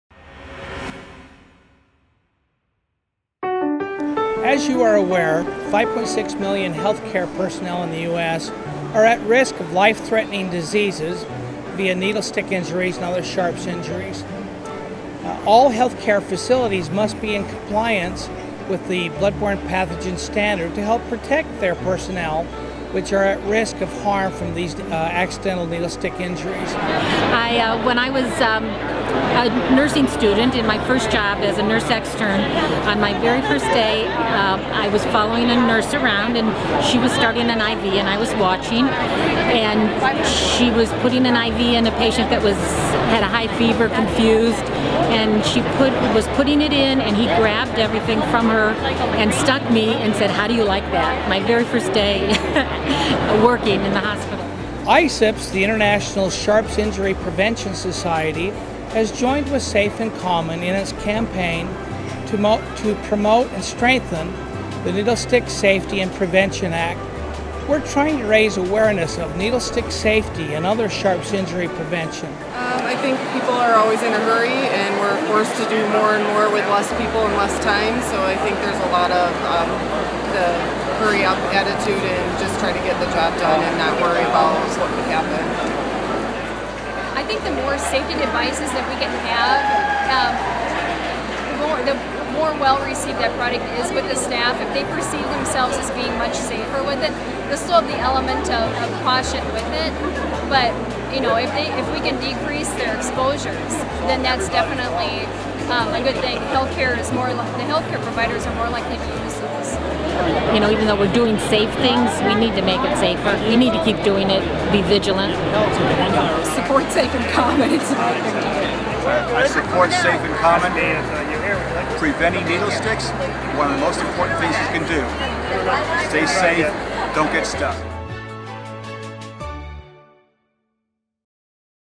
APIC Video
Safe in Common promotes needlestick safety at the annual APIC conference.